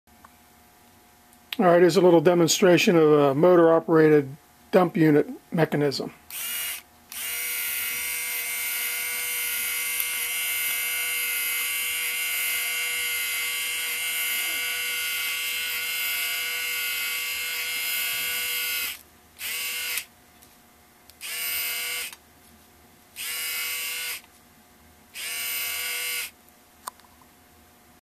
Homemade motorized dump unit mechanism.
Scratch built mechanism to operate a Marx type dump unit to be used with Marx dump cars.